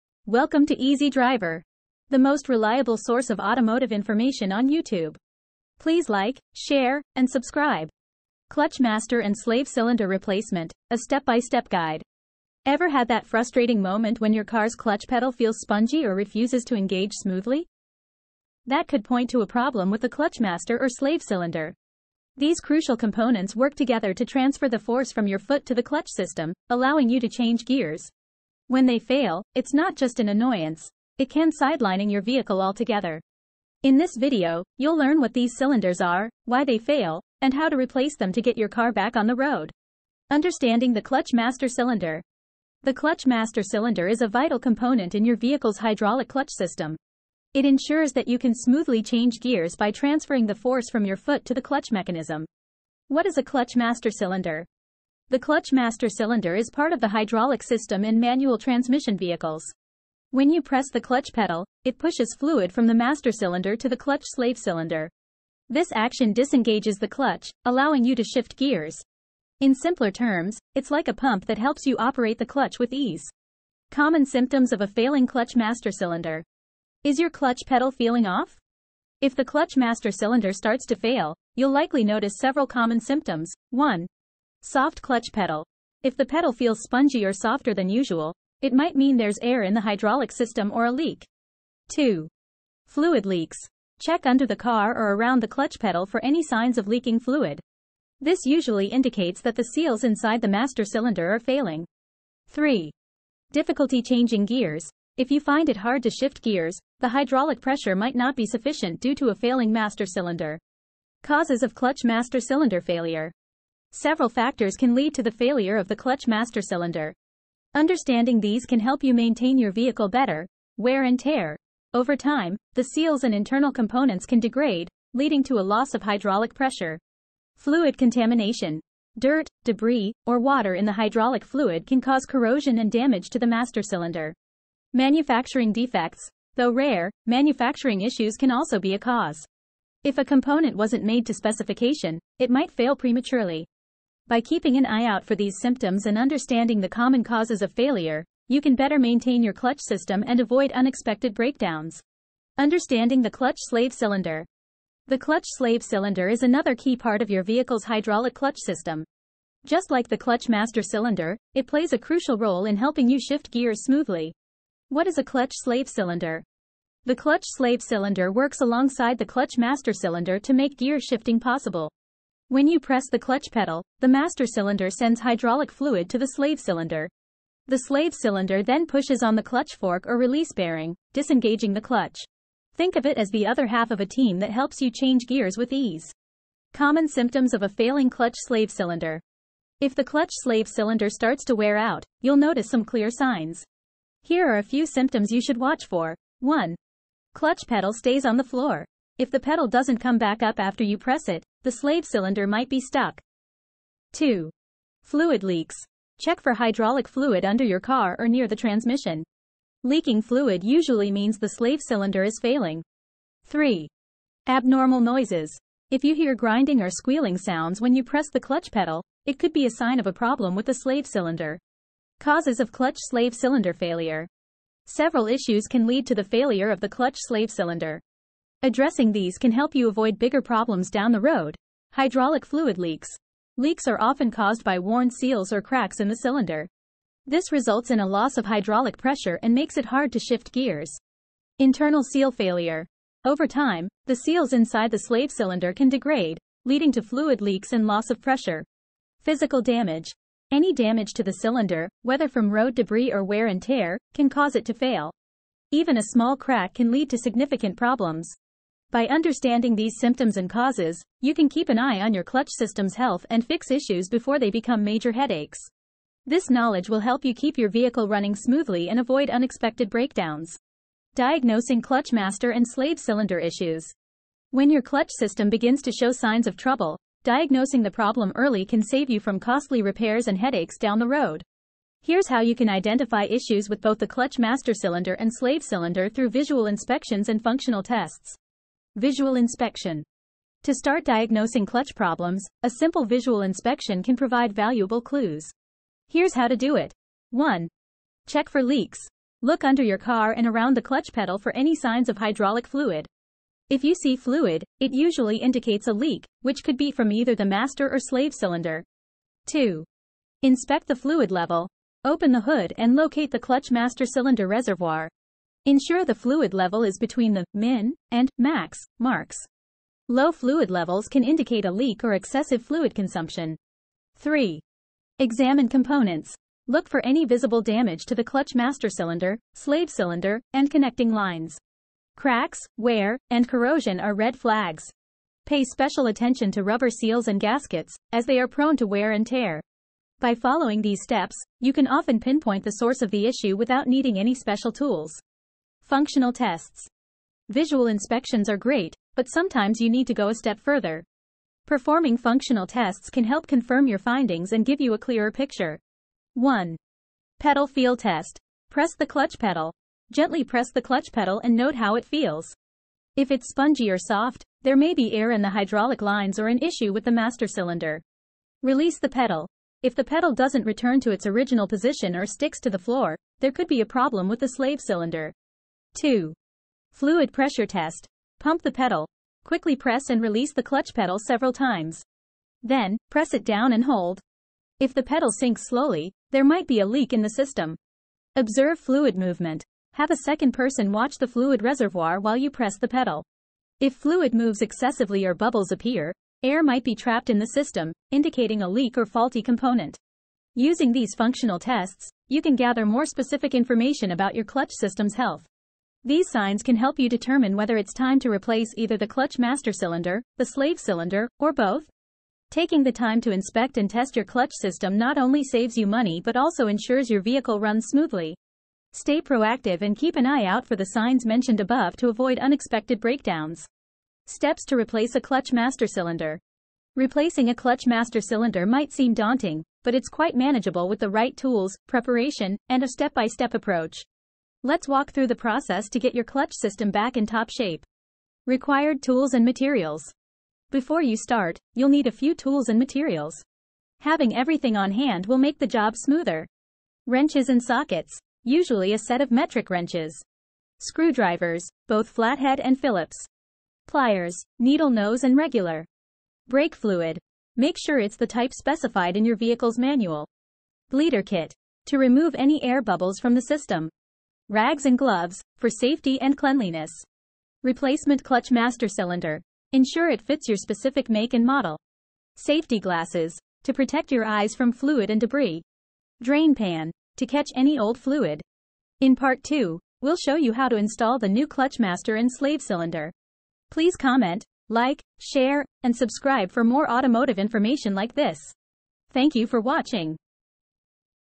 Clutch Master & Slave Cylinder Replacement: A Step-by-Step Guide | Part 1
Welcome to Part 1 of "Clutch Master & Slave Cylinder Replacement: A Step-by-Step Guide." This detailed tutorial walks you through the process of replacing your clutch master and slave cylinders with precision and confidence. Whether you're a novice or a seasoned DIYer, our comprehensive instructions and expert tips ensure you can tackle this essential repair with ease. Learn about the tools required, common challenges to anticipate, and how to troubleshoot along the way.